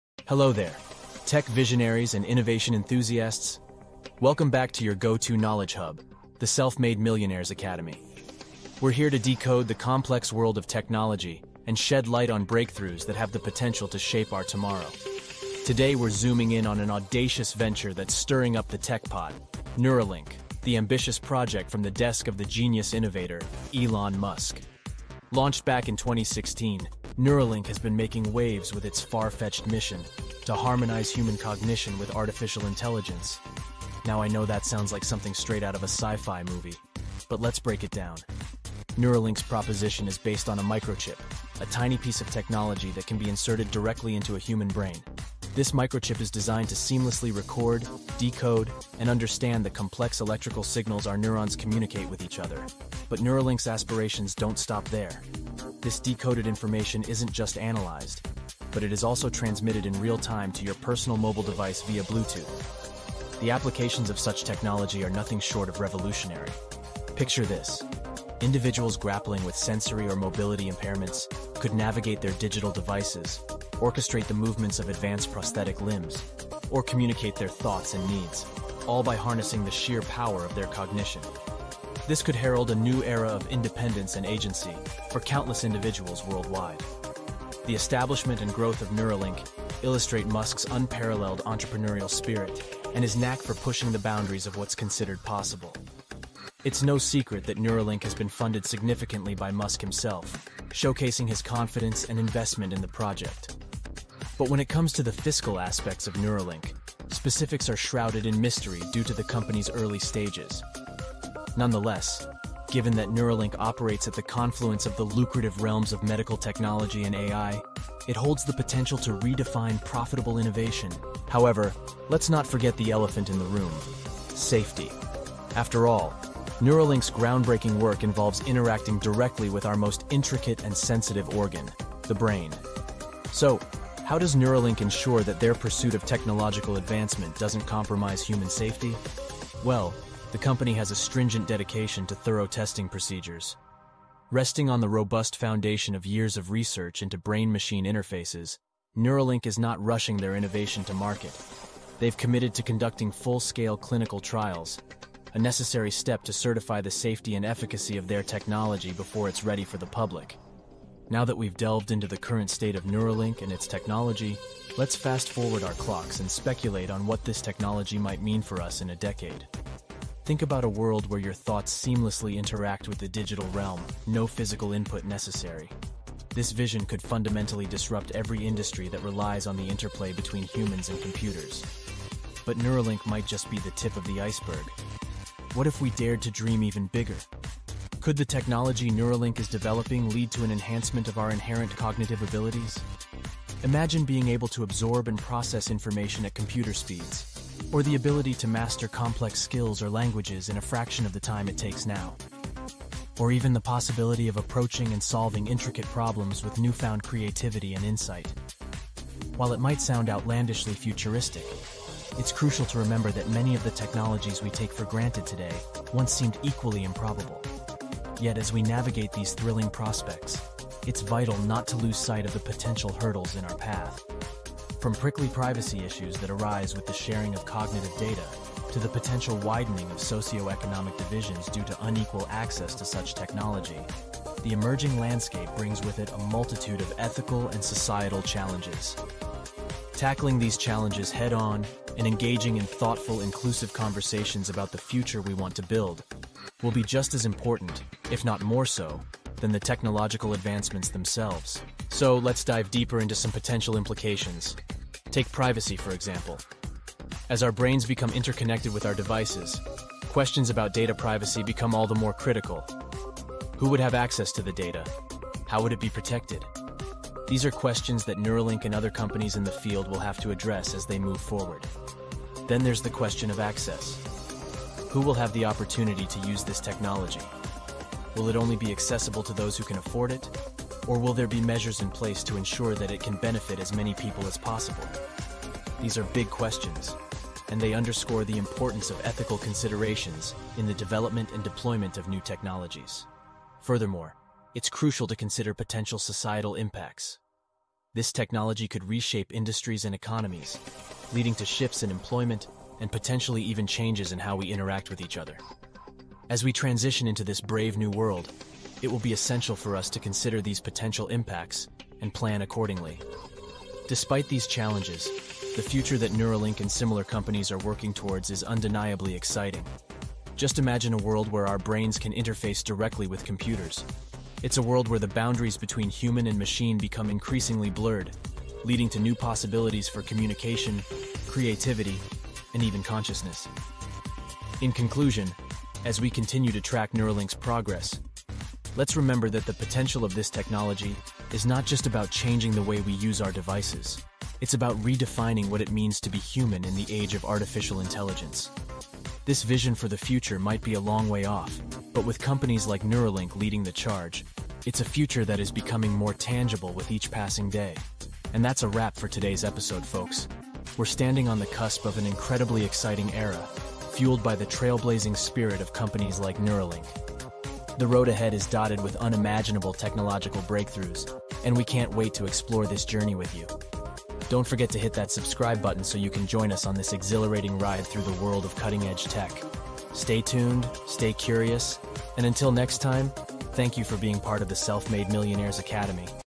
Neuralink: Unveiling Elon Musk's Mind-Blowing Brain-Computer Interface
🚀 Welcome back to Self-Made Millionaires Academy! Today, we're tackling a subject that's truly out of this world - Elon Musk's cutting-edge technology, Neuralink 🧠💡. We're diving deep beneath the surface of this groundbreaking tech, unravelling the mysteries behind this brain-computer interface that's turning the world of sci-fi into reality!

💰 Curious about the financial gears that power Neuralink? We've got you covered. Understand the complexities of its technology and the awe-inspiring future it promises. We're not just talking innovation here - we're exploring a revolutionary shift that could redefine human potential and the way we interact with our digital world 🌐💼.

🔮 Step into the extraordinary as we paint a vivid picture of a world transformed by Neuralink - a seismic shift in medical science, AI, and even human cognition. This is more than just a tech story; it's a glimpse into a future that could be just around the corner!

Stay on the pulse of tech and business evolution with us. Together, let's push the boundaries of what's possible! 💫